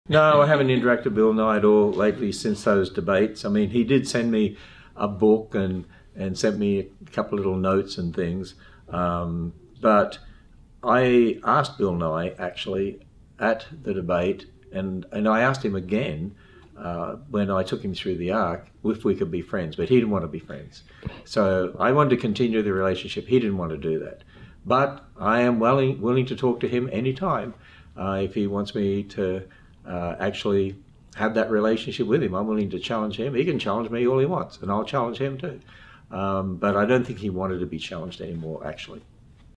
Do Billy Nye and Ken Ham Still Talk?
CBN Recently attended the Homeschooling Experience conference at the Ark Encounter in Williamstown, Kentucky last month and founder Ken Ham spent a few minutes discussing the rise in homeschooling and why it's so important for Christian parents. In this video, we asked Ken if he still interacts with Bill Nye on any level.